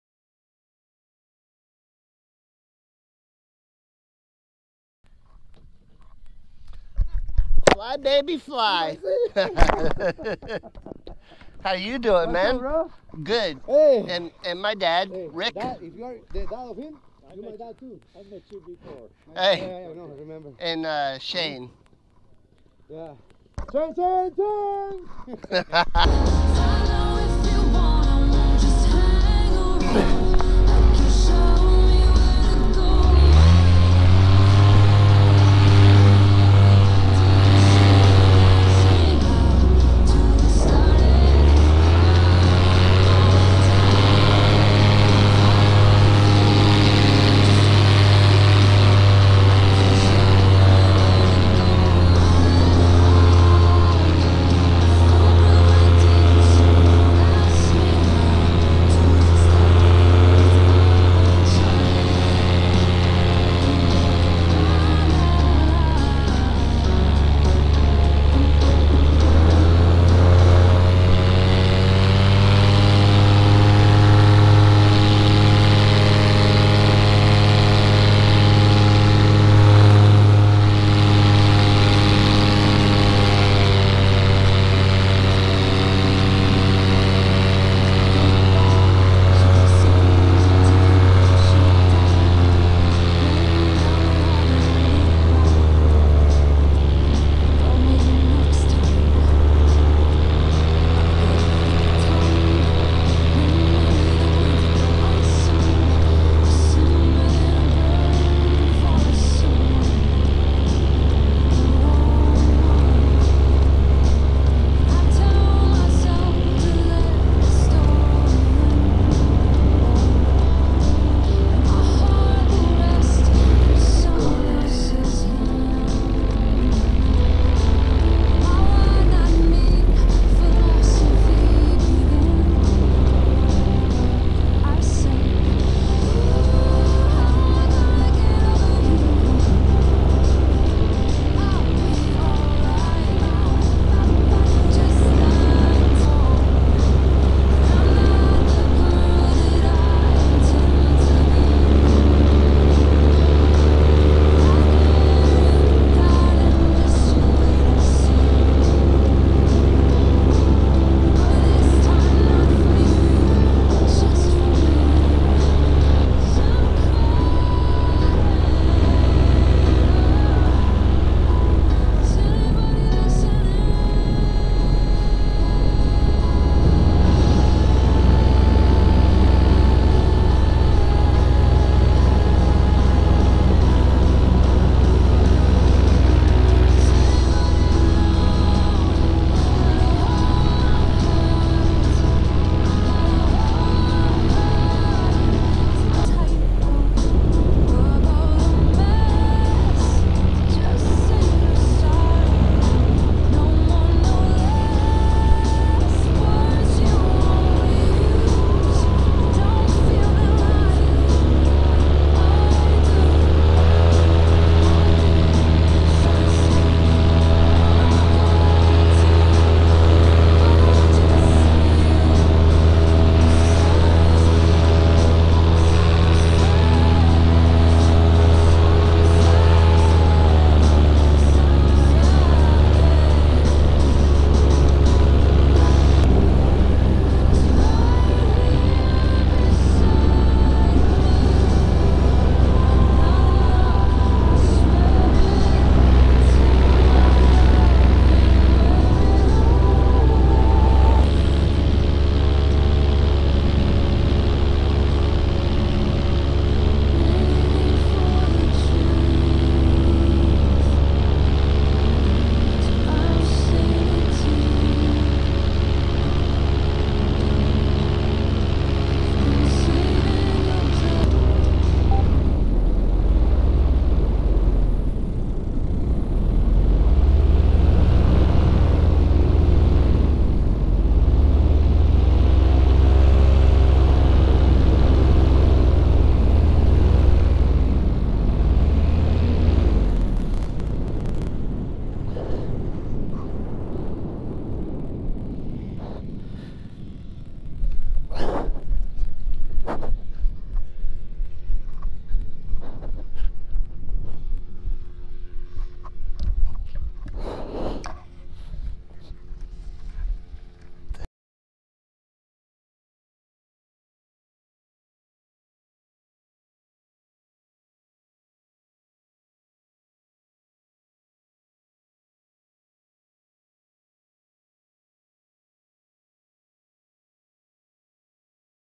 Sunniland Ranch Flyin Okeechobee Florida
Wet and windy, not much flying but we knew that before we left the house. One thirty minute flight was all I chose to get. I had a great time camping with friends, fires and food. Thanks Never Trust A Skinny Chef Shane